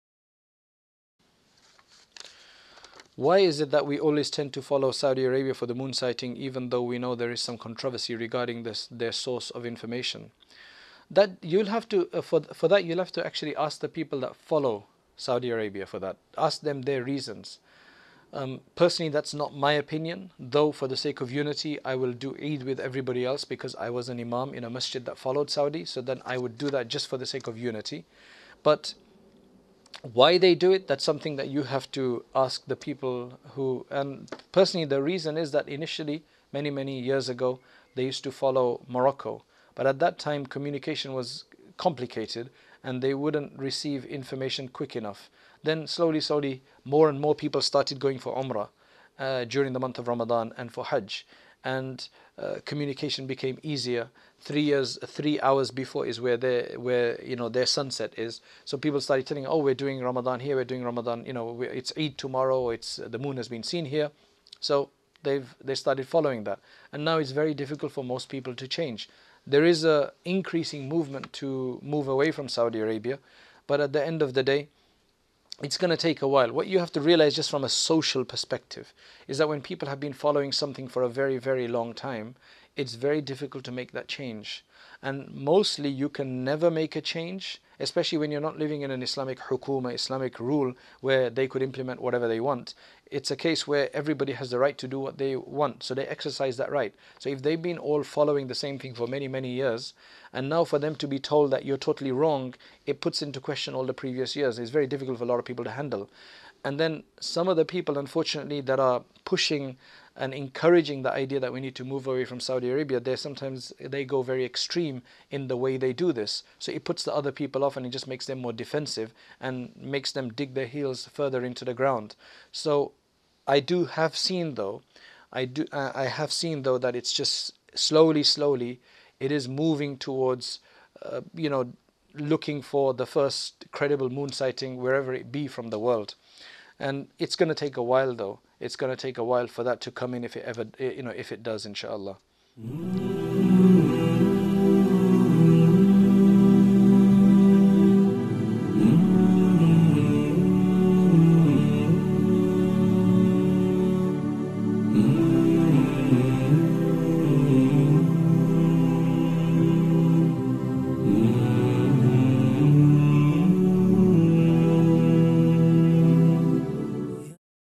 Q&A: Why do We Follow Saudi for Moon Sighting | Mufti Abdur-Rahman ibn Yusuf
Question: Why do We Follow Saudi for Moon Sighting?